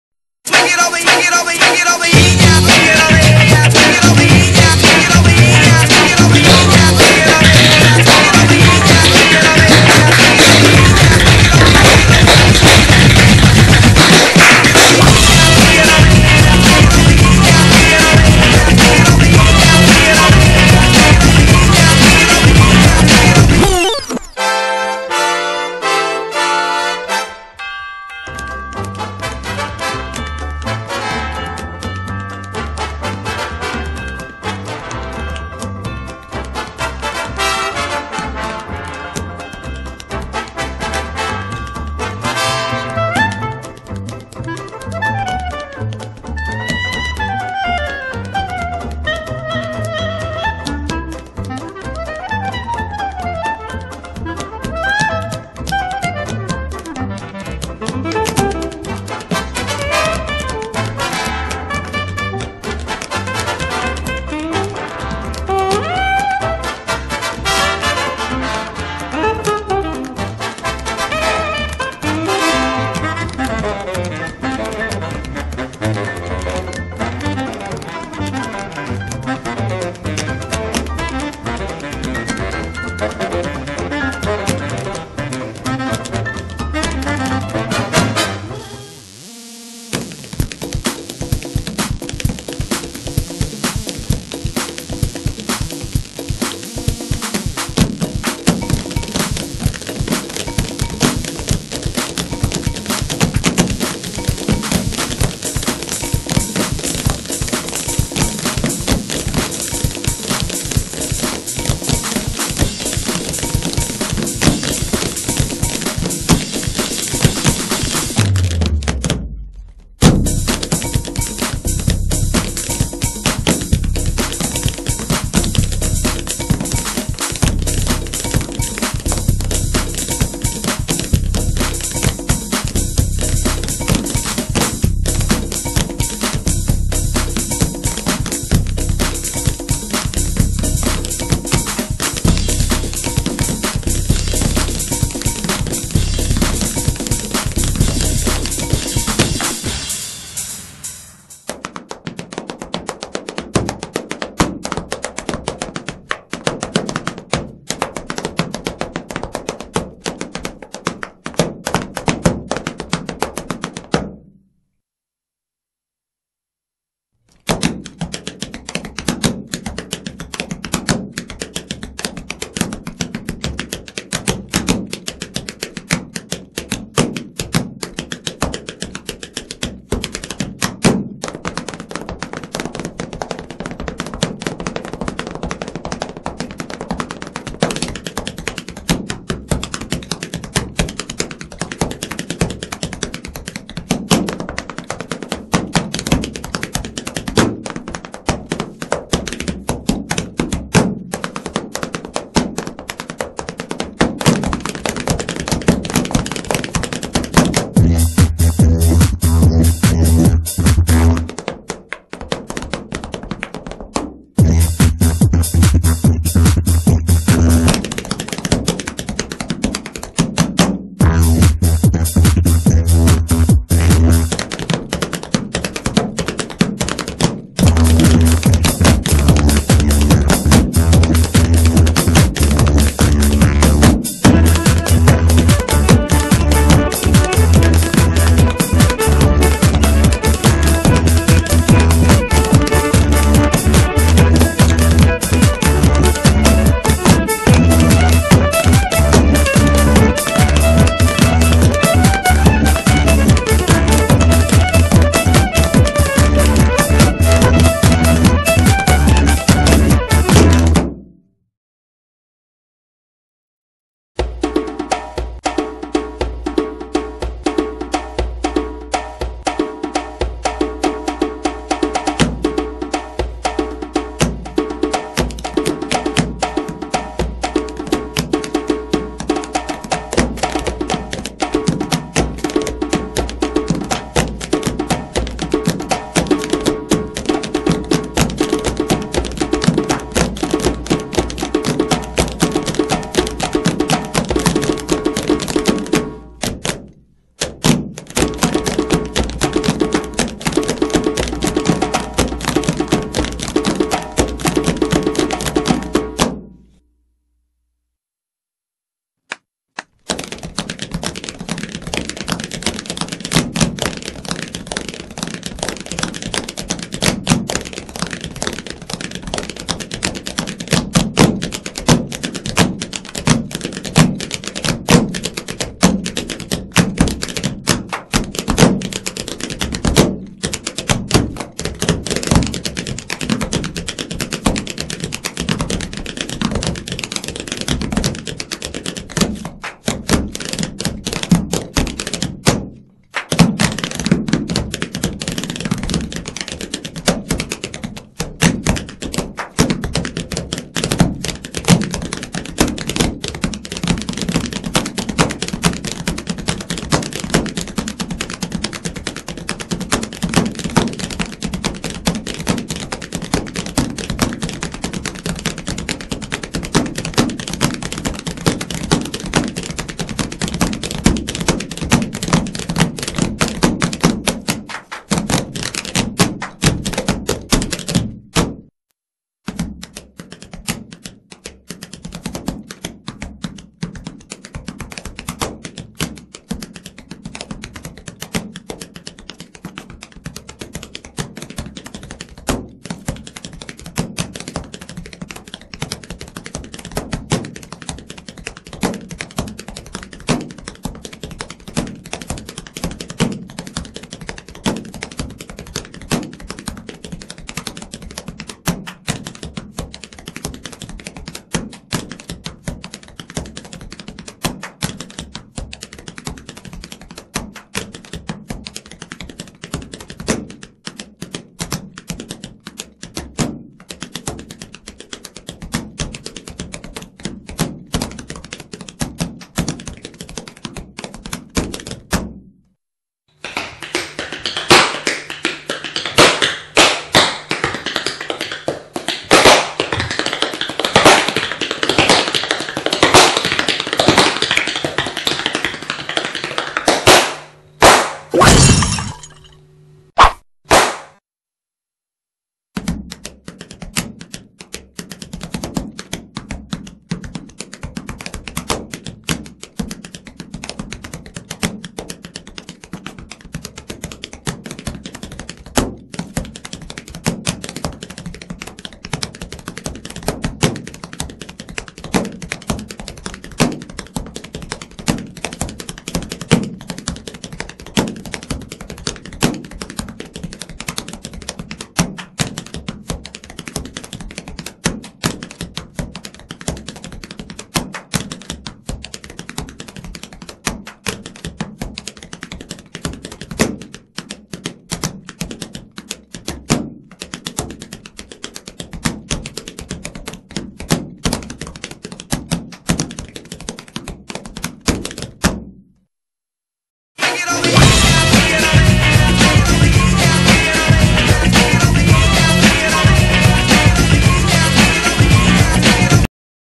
RED HOT RHYTHM
A fascinating and energetic fusion of tap dance, Irish dance and African drumming to appeal to audiences young and old, RED HOT RHYTHM is sure to impress.